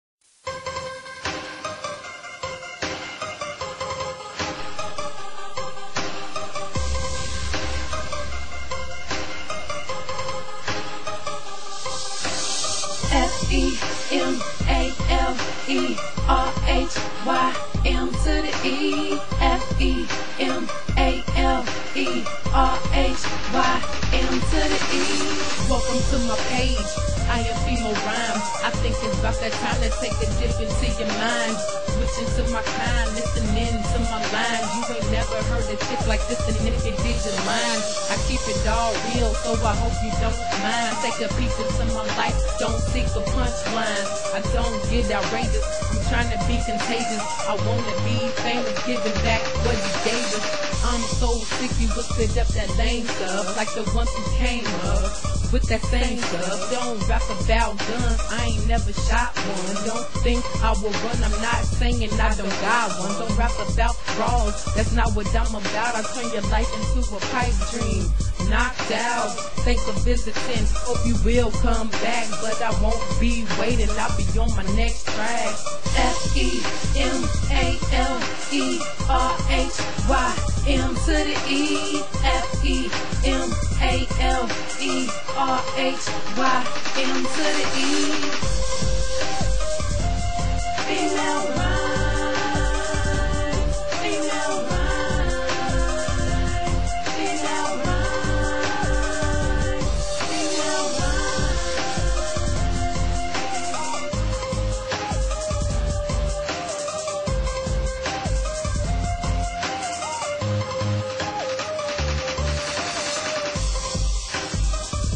Female Rhyme's "Freestyle" Video
Hey, whatsup. I have been practicing how to freestyle and today was the day that I actually made it all the way through 16 bars! I was recording myself freestylin so I made a video for it. I am messing up a little in the video because I couldn't remember everything I said..lol.. but. check me out...Peace. Beat by: JaeSwiftTV